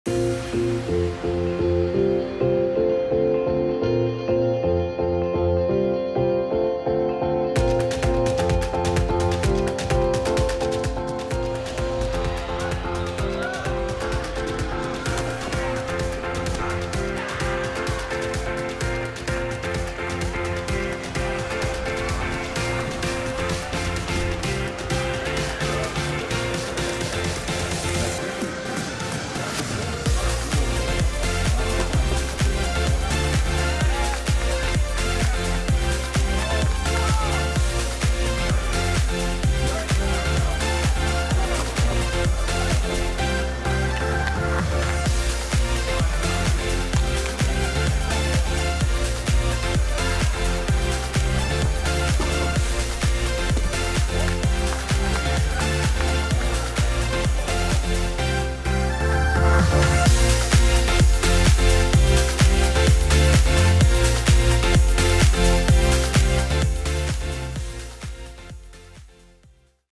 Top 5 Routines - Vault - Junior Men Day 1 - 2024 Xfinity US Championships
Dickies Arena - Fort Worth, Texas